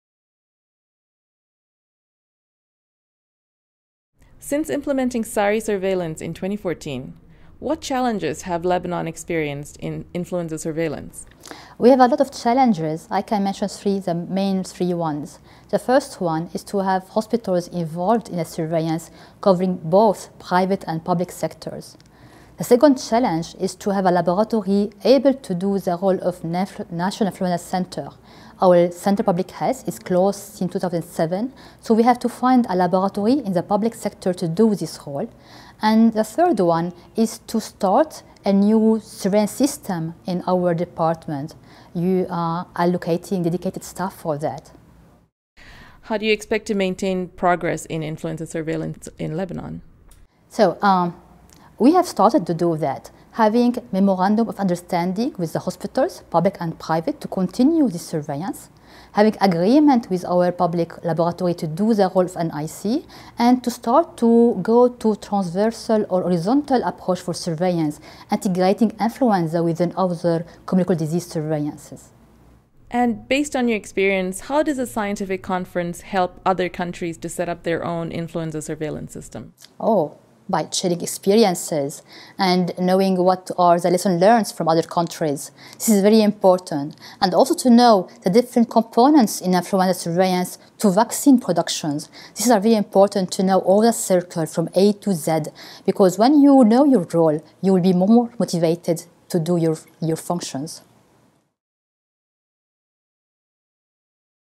A review of SARI surveillance in Lebanon – Dr Nada Ghosn
Dr Nada Ghosn from the Ministry of Public Health, Lebanon, describes the challenges and progress made in Lebanon since surveillance of severe acute respiratory infections (SARI) was implemented in the country in 2014.

The WHO Regional Office for the Eastern Mediterranean hosted the first Scientific Conference on Acute Respiratory Infections (ARIs) in Amman, Jordan from 11 to 14 December 2017. The conference was held together with the Fourth Meeting of the Eastern Mediterranean Acute Respiratory Infection Surveillance (EMARIS) Network, which is a network of countries in the Region working together to review progress made in surveillance and response capacities for seasonal and pandemic influenza.